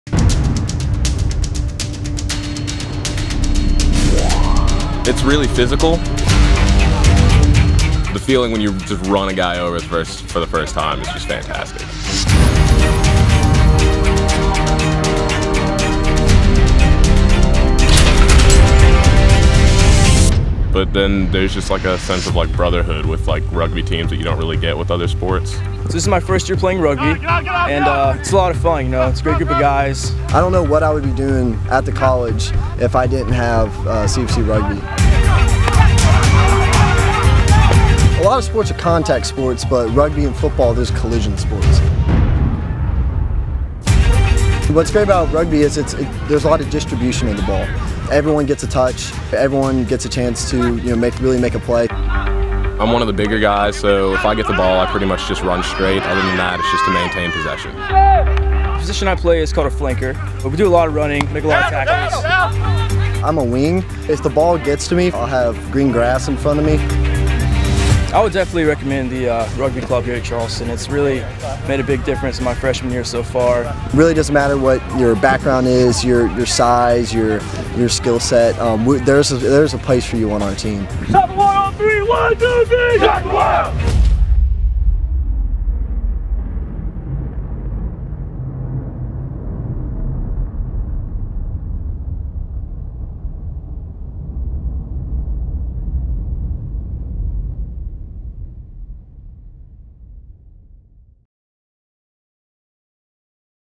College of Charleston Men's Rugby Football Club
The College of Charleston Men's Rugby Football Club was founded in 2002. Since then, the club has steadily grown into a powerful club in the southeast. As part of the newly restructured USA Rugby Southern Conference Coastal Division, the Cougars are in one of the most competitive districts in the country. Their Matrix opponents are The Citadel, UNC-W, and Coastal Carolina.